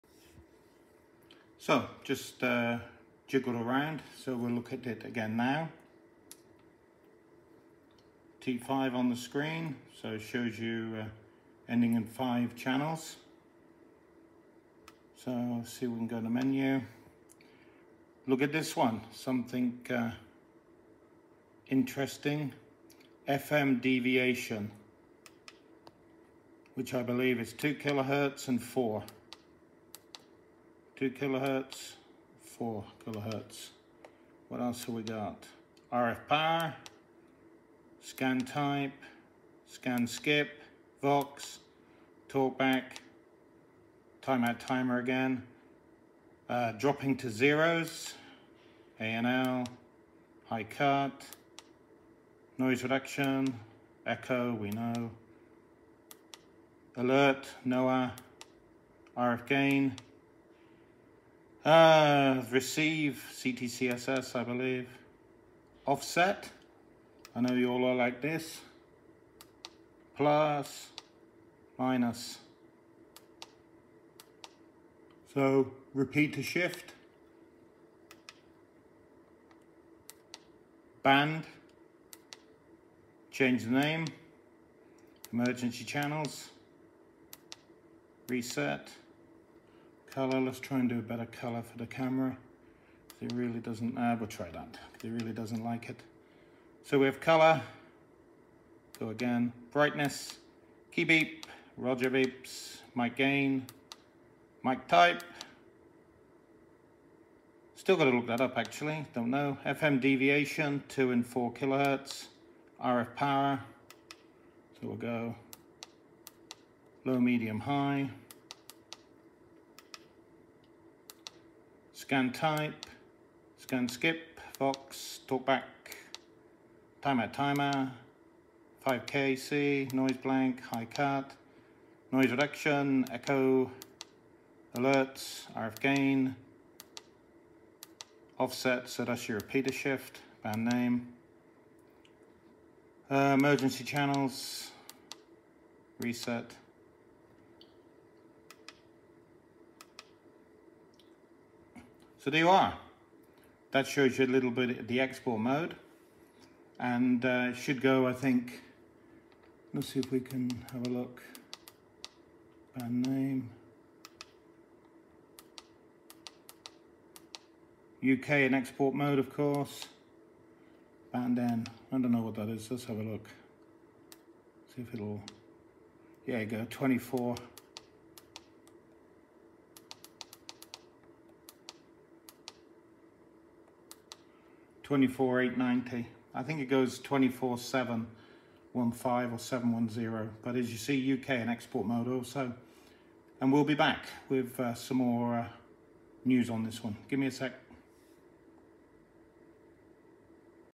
Radioddity CB-500 “Export Mode” Settings
So the radio bottoms out at 24.710 and happily goes to the top at 30.110 on both AM and FM. And look at the menu what an array of goodies including CTCSS and DCS, echo, repeater shift and of course the most interesting of all FM Deviation which allows both 2khz and 4KHz bandwidth. There is quite a lot of things happening inside this radio and we will show you more on the blog at the earliest time! It’s only landed here around 10 minutes from the courier so wait and we will be back with more. And YES enhanced EU mode is available also.